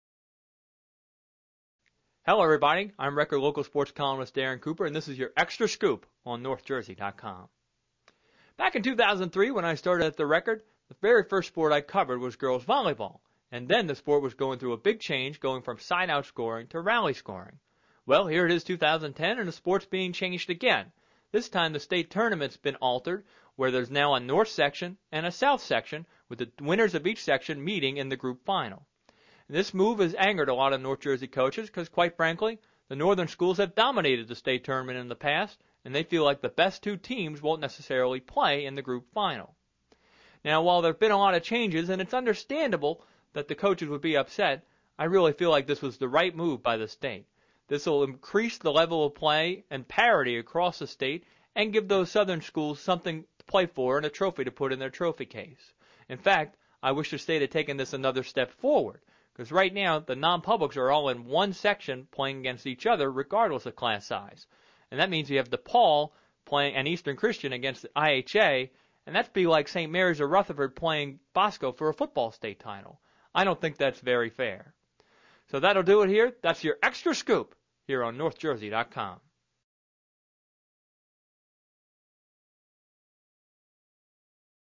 North Jersey Local Sports Extra Scoop
Changes to girls' volleyball state tournament rules for New Jersey high school athletes are discussed by Record columnist Darren Cooper.